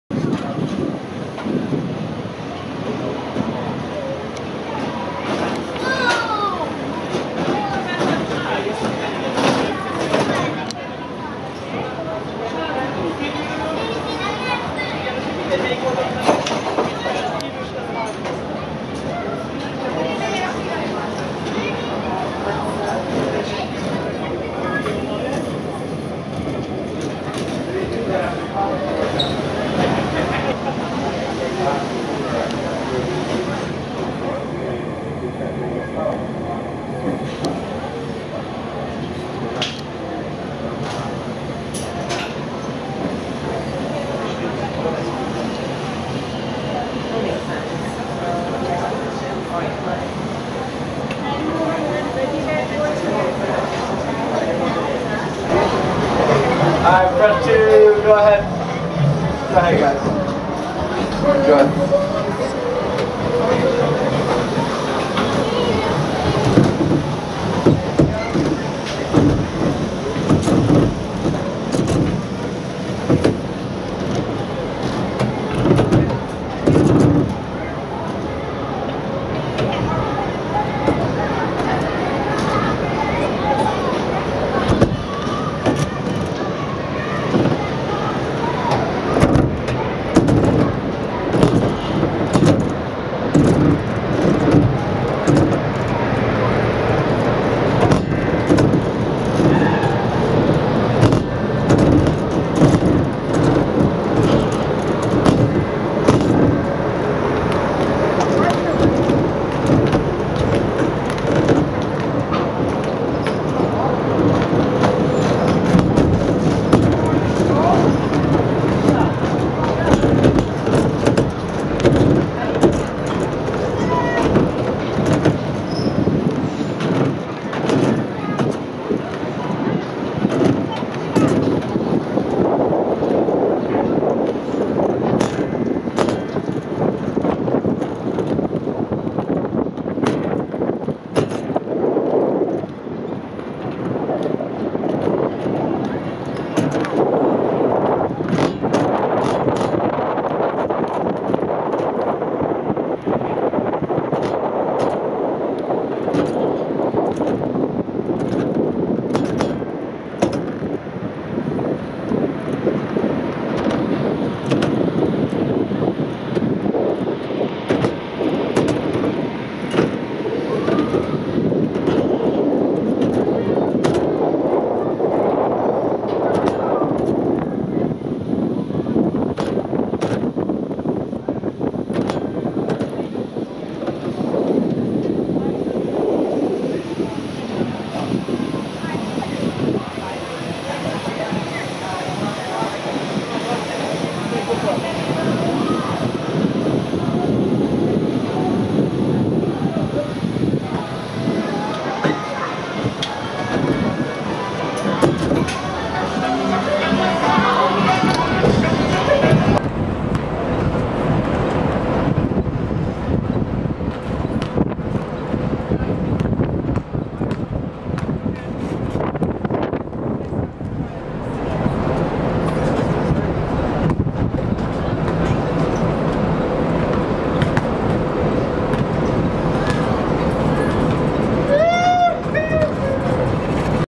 Sky Ride - Carnival Vista
The Sky Ride is sort of a bicycle, hanging on tracks where you can cycle around the sports deck of the Carnival Vista cruise ship - great fun!